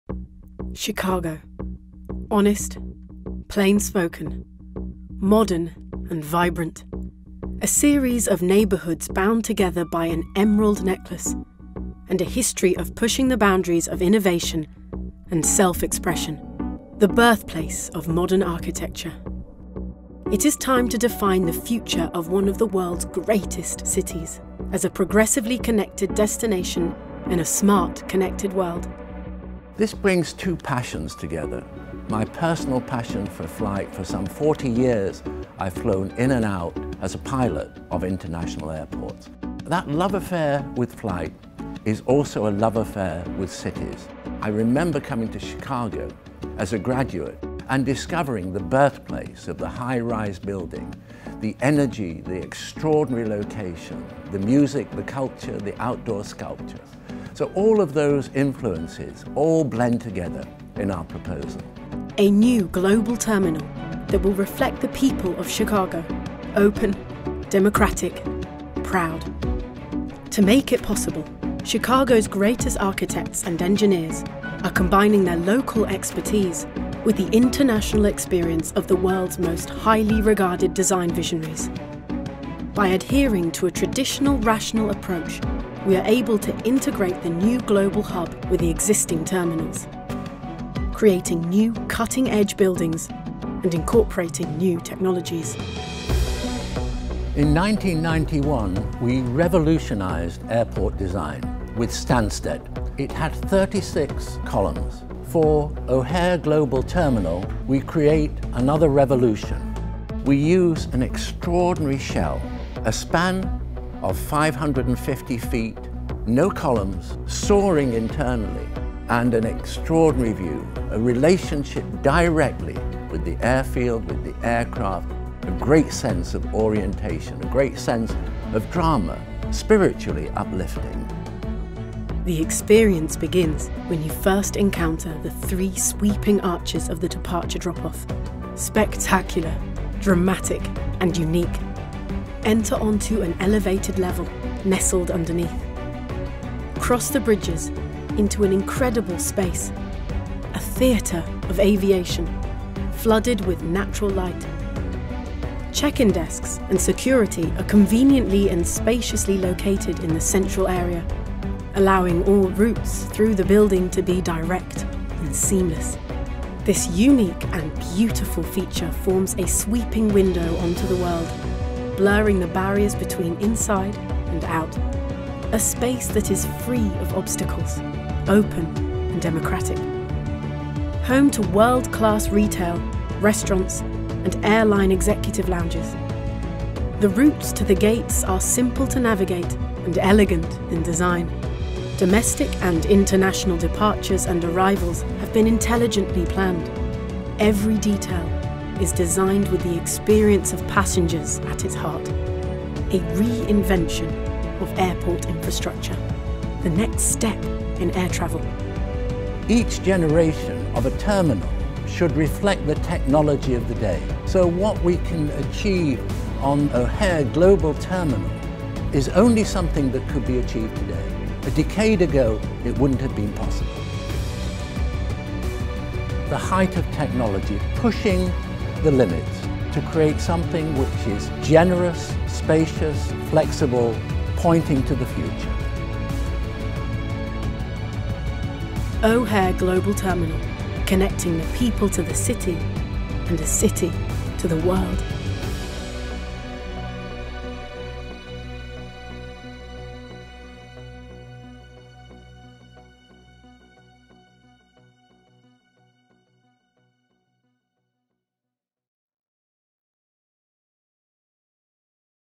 Chicago's O'Hare Global Terminal, led by Foster + Partners, Epstein and Moreno
Foster Epstein Moreno, has created a dynamic and iconic design for O’Hare’s new Global Terminal, aiming to recapture the romance associated with air travel while reflecting the city’s cultural diversity. The team has partnered with local design and engineering talent such as the Rebuild Foundation, a Chicago-based organization that directly engages in the production of innovative arts, crafts and trades programs, founded and led by artist Theaster Gates, to work on this incredible project. With a team that is grounded in the city and backed by global expertise, we understand that Chicago is a world player, but one that doesn’t forget about its backyard.